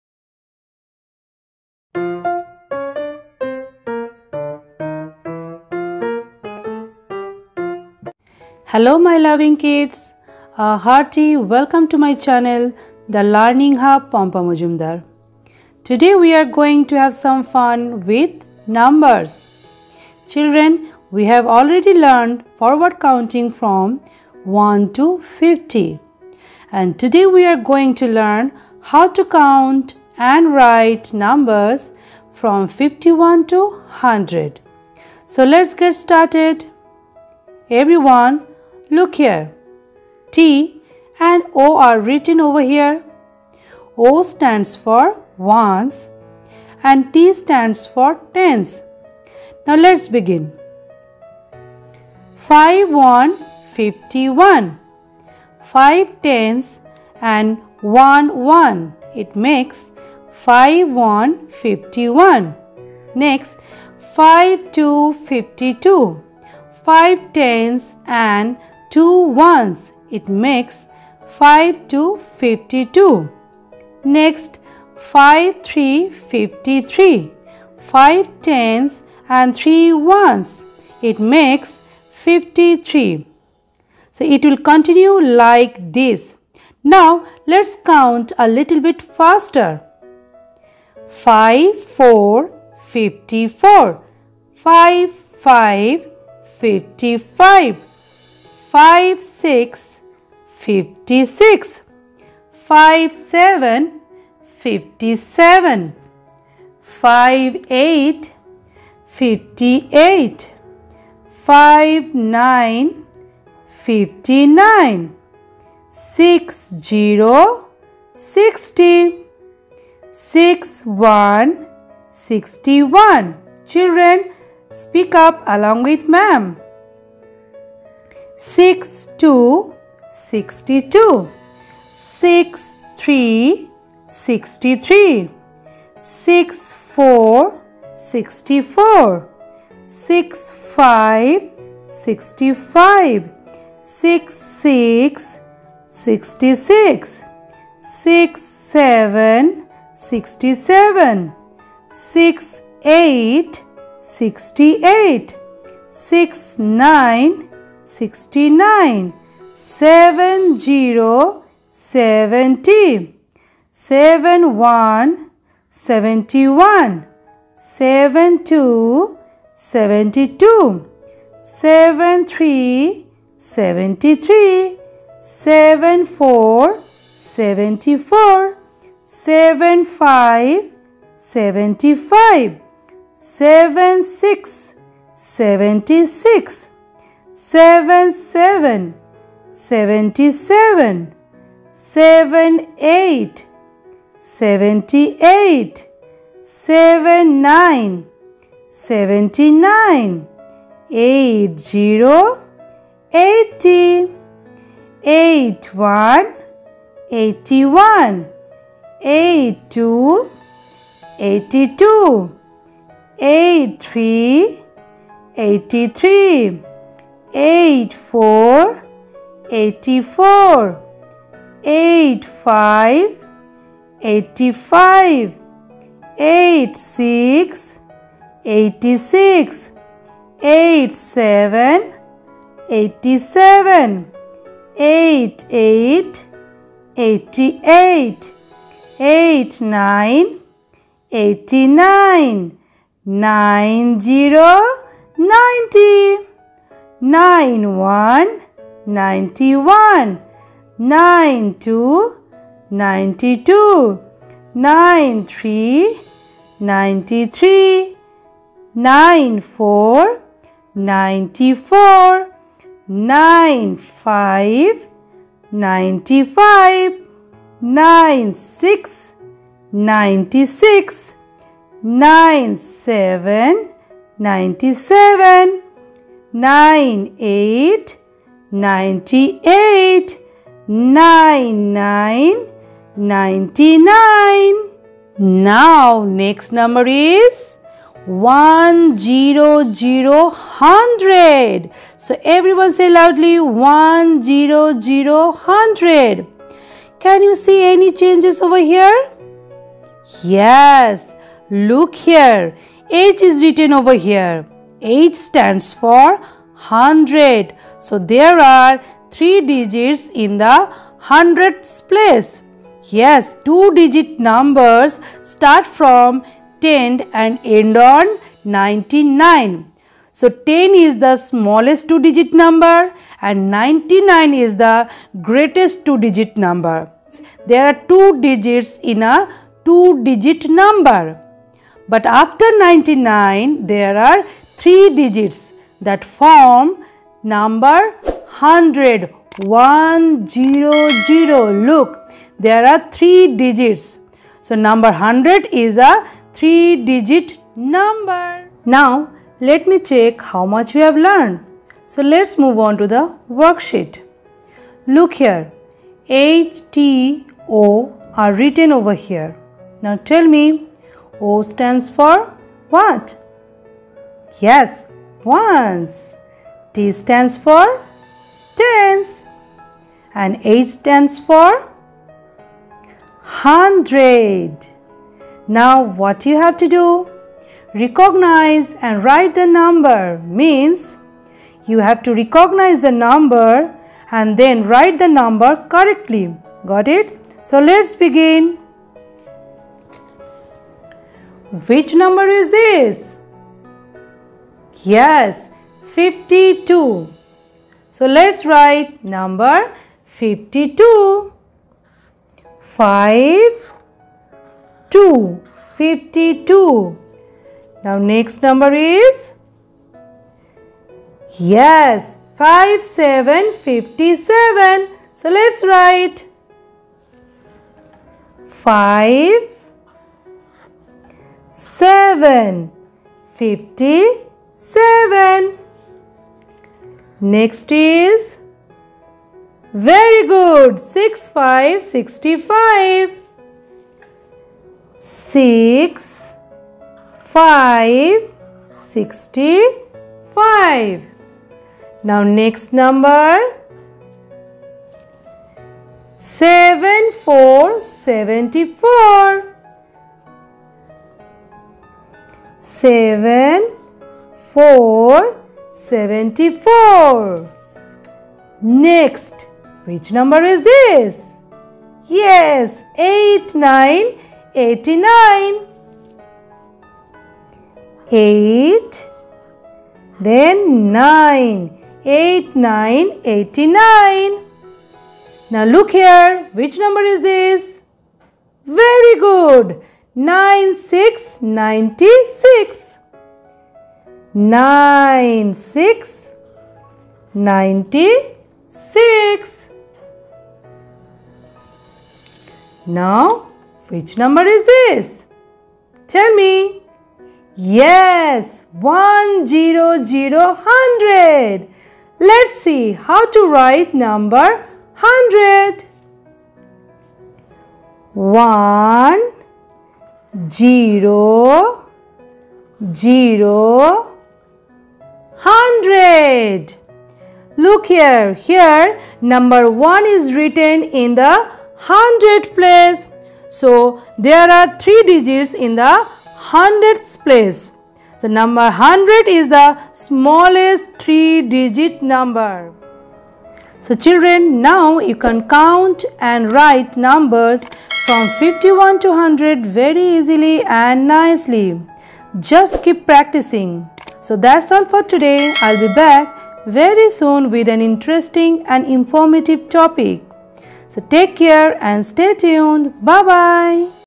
Let's learn counting 51 to 100/forward counting 51 to 100
counting from 51 to 100..It's a very easy topic despite of this I will request you to watch the video ...

previous videos:-